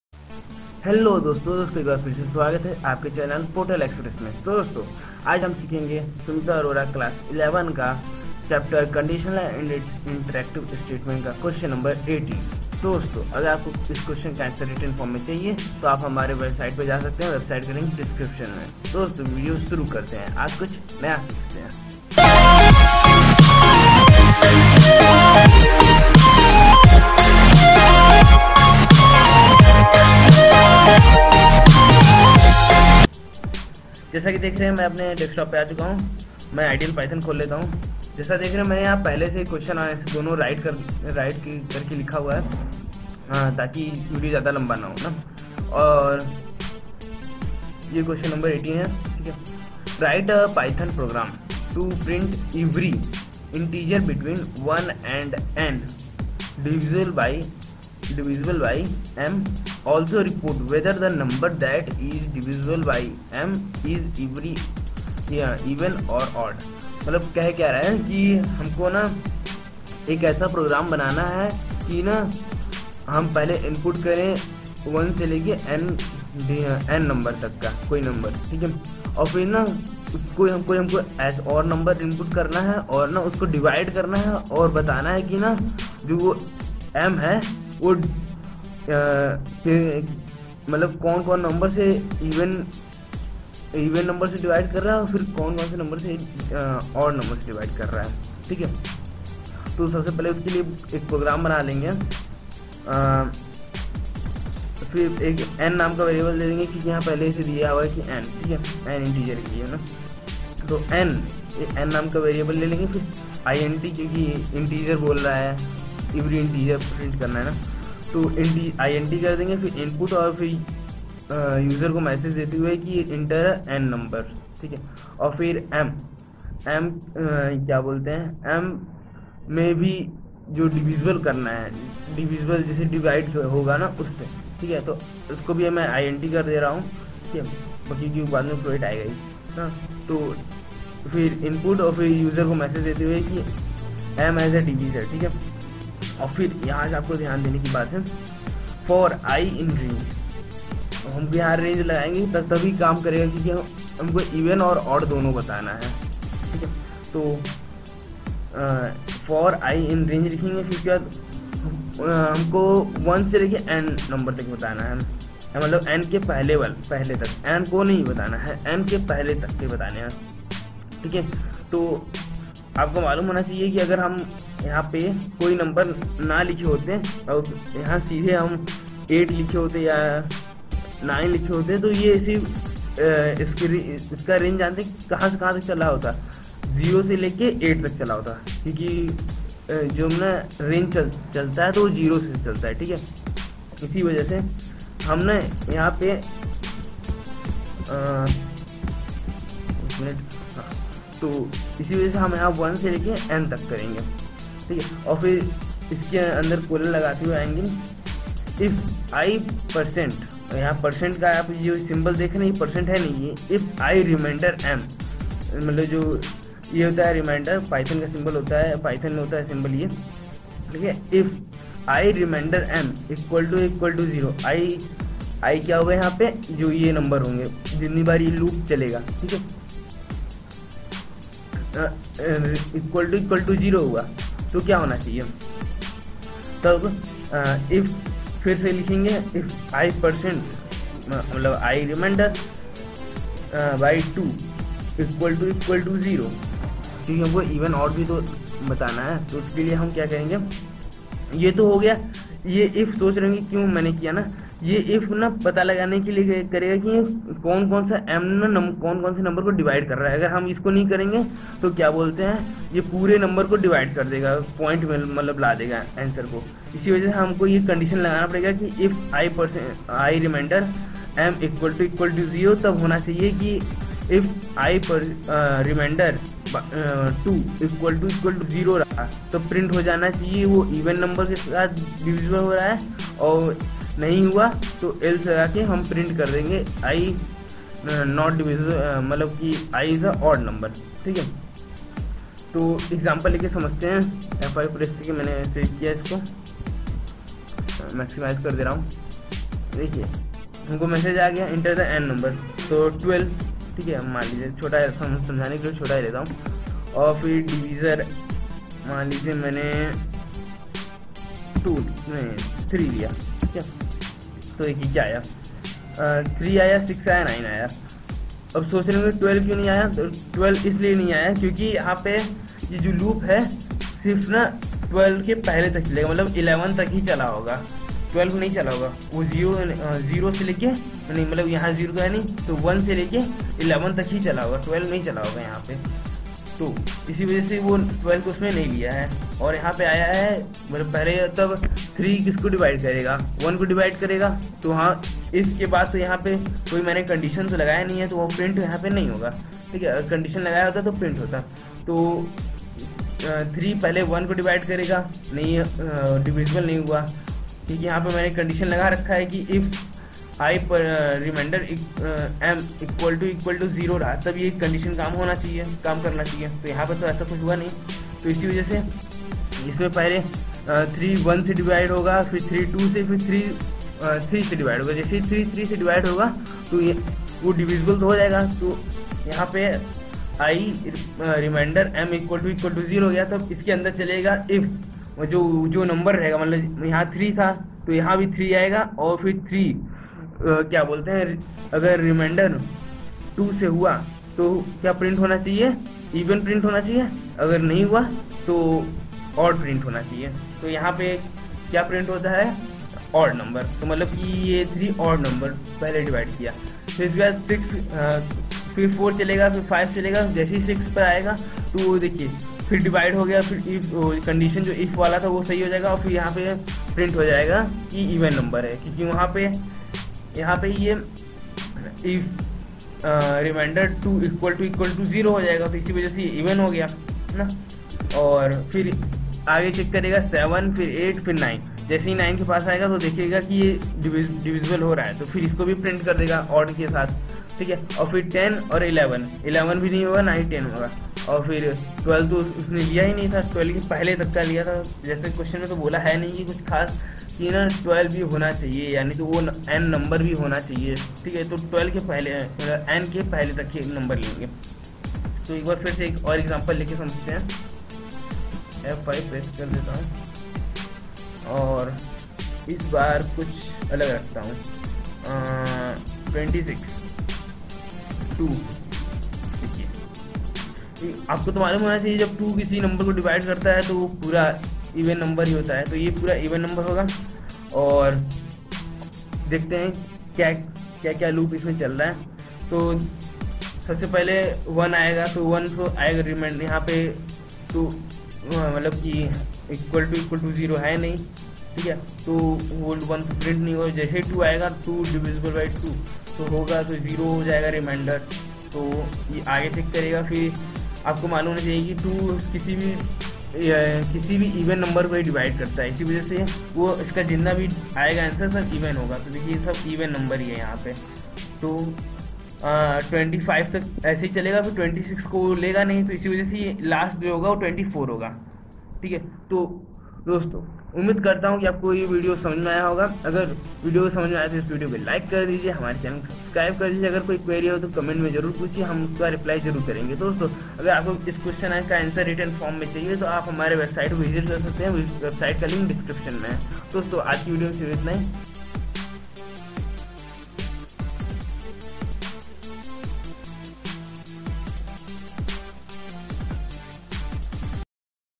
Write a python program to print every integer between 1 and n divisible by m. also report whether
Sumita Arora Class 11 computer science chapter conditional and iterative statement type C Q 18||
Q. Write a python program to print every integer between 1 and n divisible by m. also report whether the number that is divisible by m is even or odd.

Answer :-

n = int(input("Enter n = "))
m = int(input("Enter m as divisor = "))
for i in range(1,n):
    if i % m == 0 :
        if i % 2 == 0 :
            print(i,"is divisible by m and this is even number ")
        else :
            print(i,"is divisible by m and this is odd number ")

Output :-

Enter n = 10
Enter m as divisor = 2
2 is divisible by m and this is even number
4 is divisible by m and this is even number
6 is divisible by m and this is even number
8 is divisible by m and this is even number

Enter n = 15
Enter m as divisor = 3
3 is divisible by m and this is odd number
6 is divisible by m and this is even number
9 is divisible by m and this is odd number
12 is divisible by m and this is even number

class computer science definition,
11thclasscomputer science,